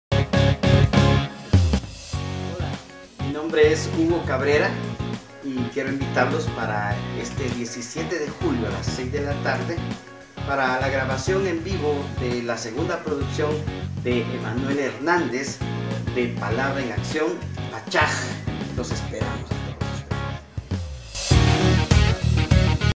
Pastor Hugo
Pastor Hugo Cabrera de Iglesia "Mi gran Familia"  invitando a Grabación en Vivo